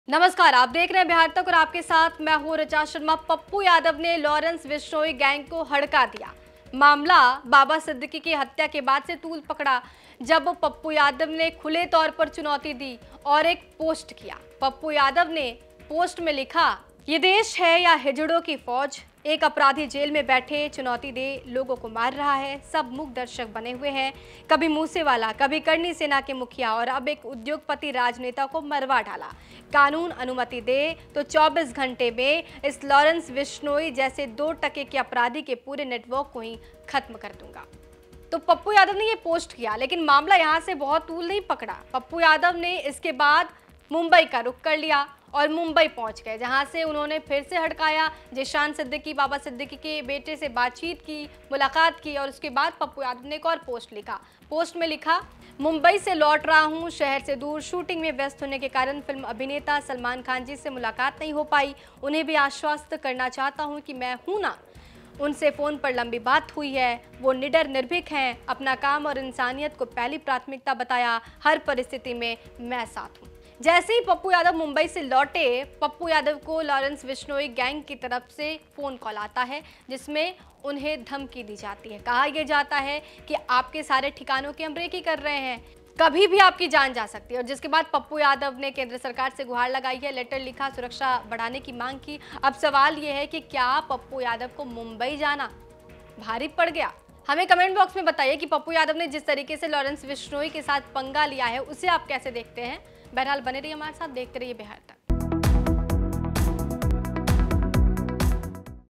Lawrence Bishnoi के खिलाफ क्या Mumbai जाना Pappu Yadav को पड़ा महंगा, अब क्या करेंगे ? Bihar Tak
Lawrence Bishnoi के खिलाफ क्या Mumbai जाना Pappu Yadav को पड़ा महंगा, अब क्या करेंगे ?